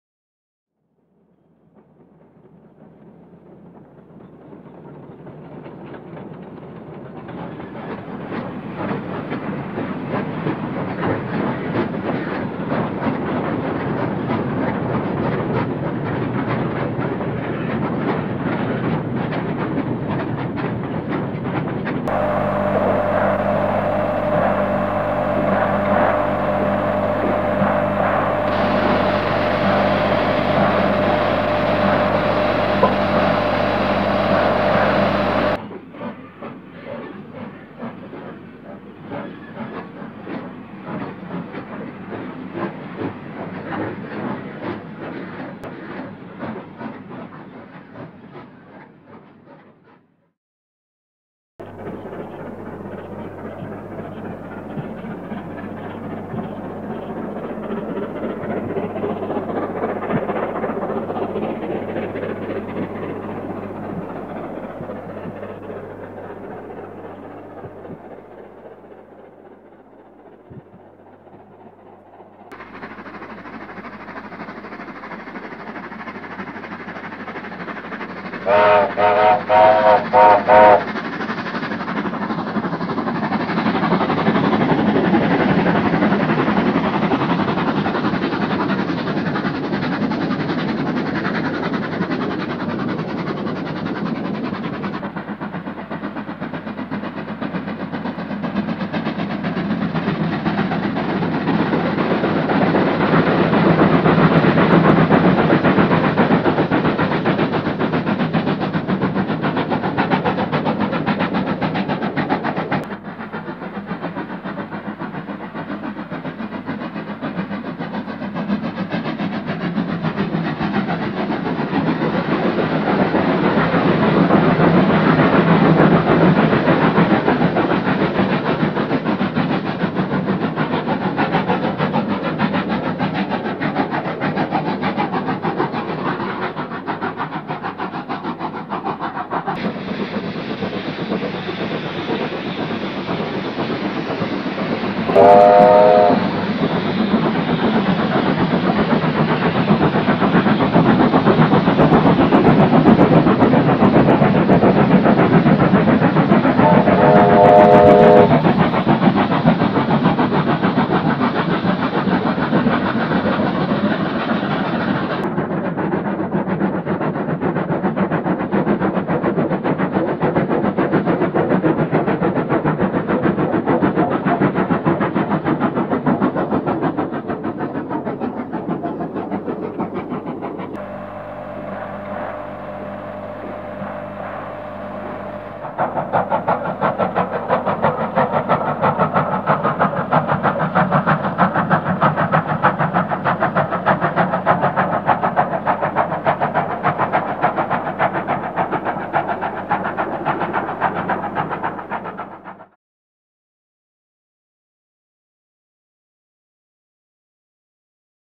3501and 3526, Blue Mountains double header, 1968.
3501 and 3526 double head up the Blue Mountains, west of Sydney, in the Winter of 1968.
The last run of double 35s.